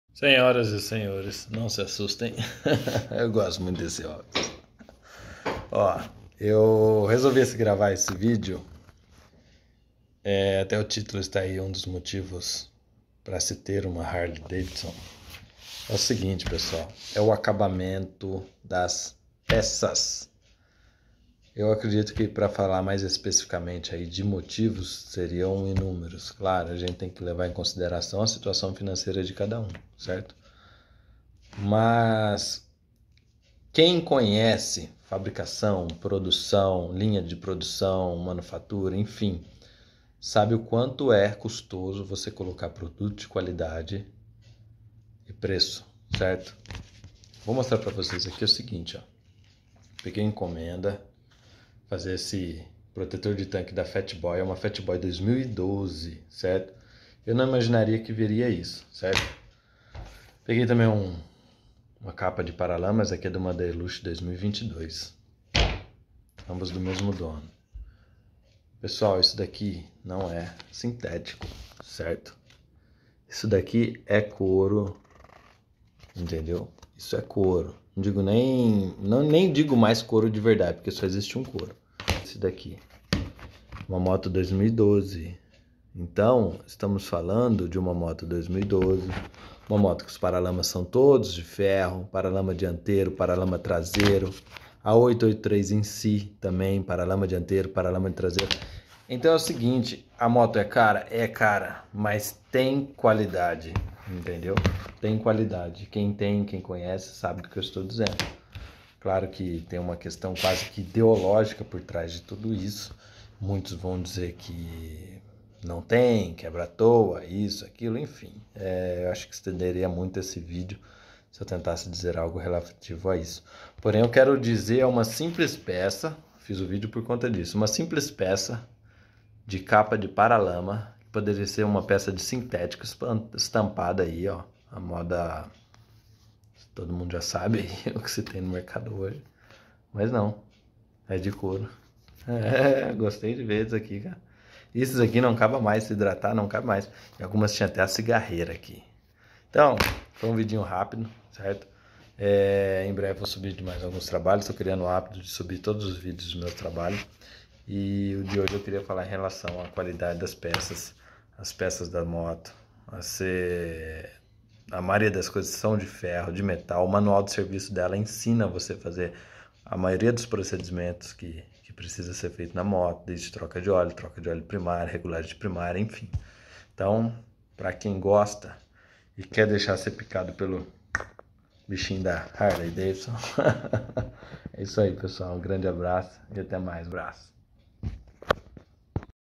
UM DOS PRINCIPAIS MOTIVOS DE COMPRAR UMA HARLEY DAVIDSON.
Olá pessoal os motivos são inúmeros porém, este me chamou muito atenção. Um item, um detalhe na verdade que eu não imaginaria, ser também de qualidade. Podemos listar inumeris, des de tecnologia no tratamento térmico das peças, por ser refrigerada a ar, e trabalhar em uma temperatura altíssima, até detalhes como esse. Harley é da hora. grande abraço a todos

💀 Peças para quem vai mais longe 💀